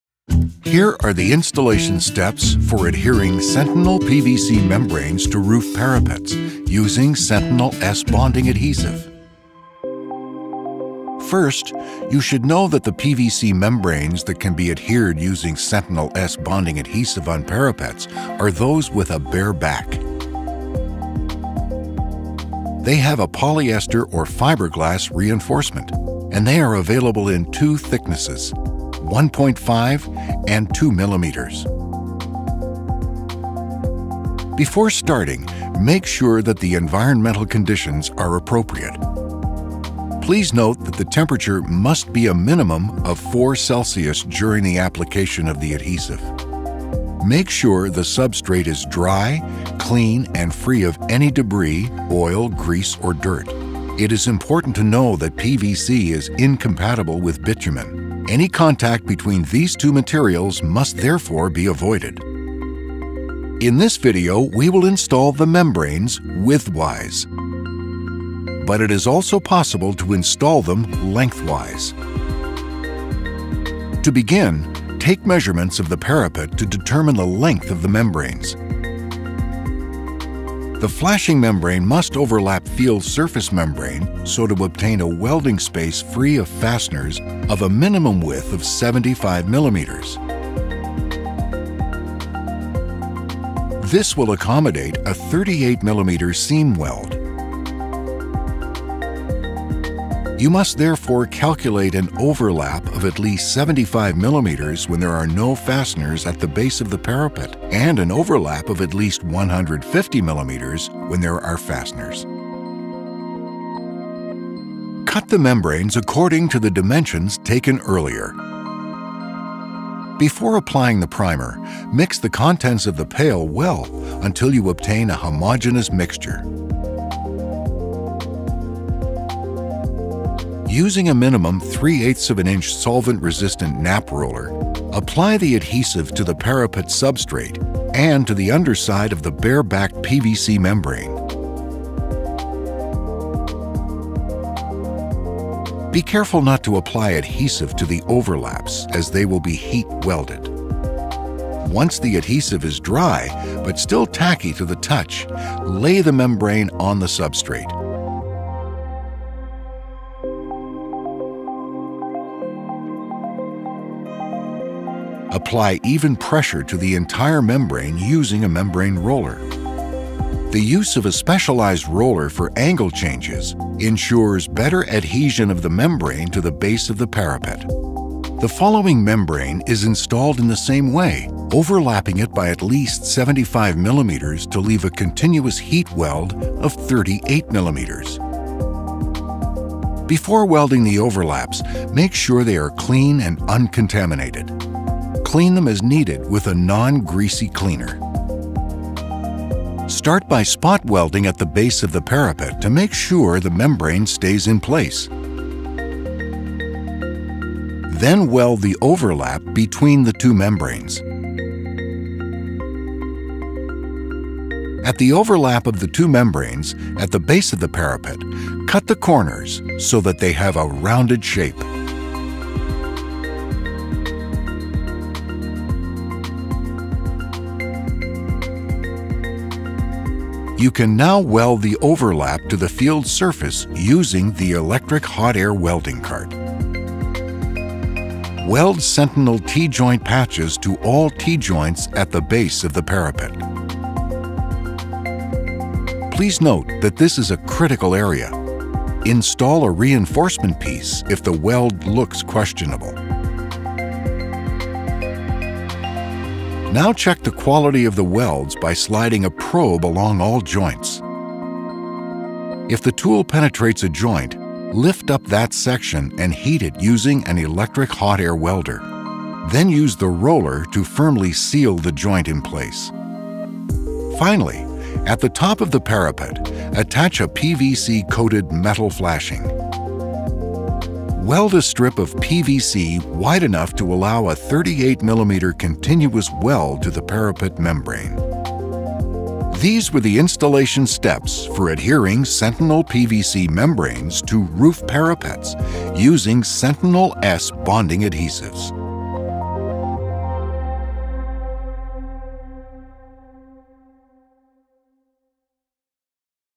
How to adhere PVC membranes on the flashing surfaces of a roof using SENTINEL S BONDING ADHESIVE?
The purpose of this video is to illustrate SOPREMA's recommendations for applying our SENTINEL PVC product line.